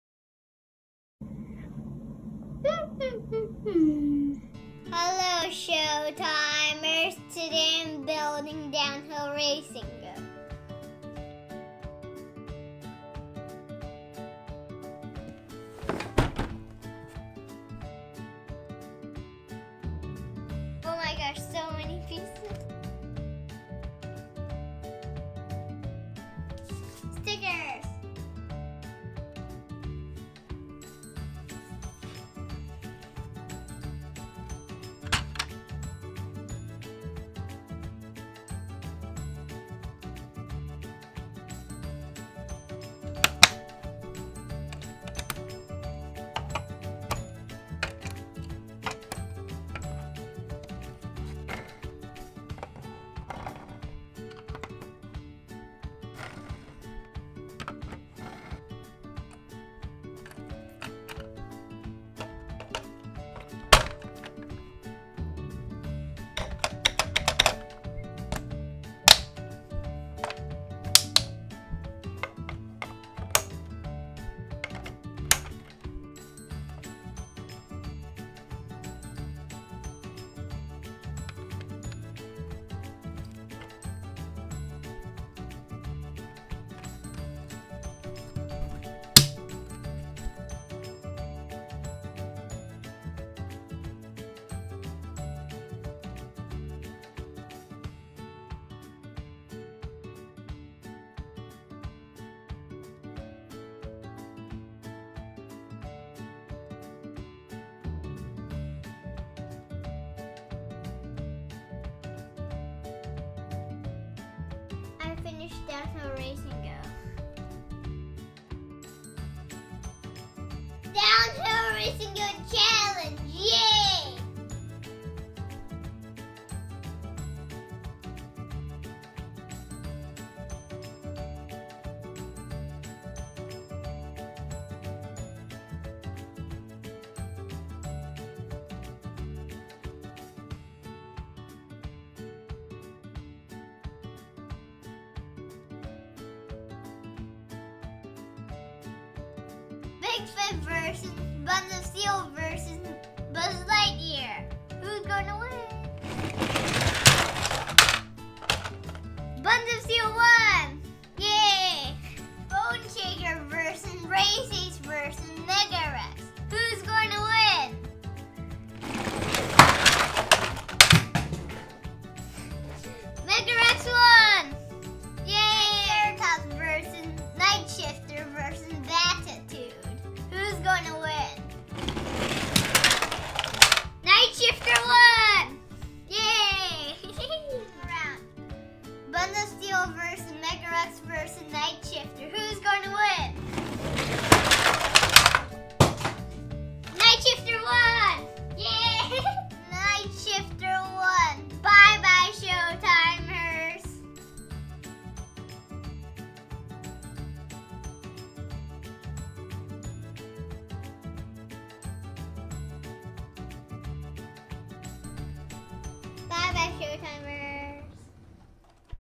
How To Build Hot Wheels Monster Trucks Downhill Race & Go 🛻 モントラのコース作り方と遊び [Track Builder]
Welcome to Trackbuilder, Showtimers!

Kids, DO try this at home!
Watch and learn from Master Trackbuilder Show as he creates some of the most excitingly original racecourses to ever be made out of orange track!

Featured Track/Playset:
Hot Wheels Monster Trucks Downhill Race & Go

Featured Course:
Downhill Race & Go

Timeline:
00:00 - Intro
00:10 - Unboxing Hot Wheels Monster Trucks Downhill Race & Go
00:30 - Applying Stickers to Hot Wheels Monster Trucks Downhill Race & Go
00:40 - Building Hot Wheels Monster Trucks Downhill Race & Go
02:00 - Playing with Hot Wheels Monster Trucks Downhill Race & Go
02:30 - Bigfoot vs Buns of Steel vs Buzz Lightyear
02:45 - Bone Shaker vs Race Ace vs Mega Wrex
02:55 - Triceratops vs Night Shifter vs Battitude
03:10 - Buns of Steel vs Mega Wrex vs Night Shifter
03:25 - Building the Hot Wheels Monster Trucks Race & Go Hauler
03:35 - Outro

Hot Wheels
Matchbox
Tomica